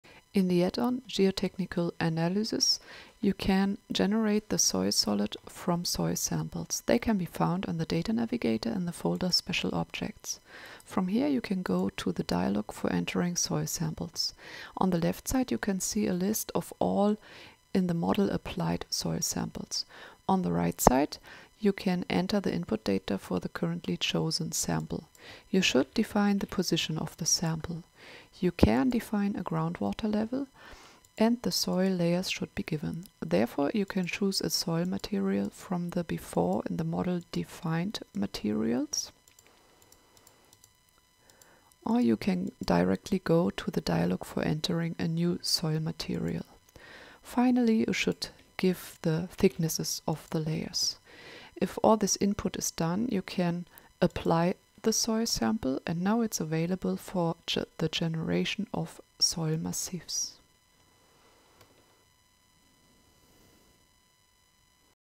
How to Define Soil Samples in RFEM 6
This video shows how you can define Soil Samples within the Geotechnical Analysis add-on. After the definition of Soil Samples, they can be used for the generation of the Soil Solids to be analyzed, the so-called Soil Massifs and its Layers.

The best way to explore RFEM is to simply test it yourself.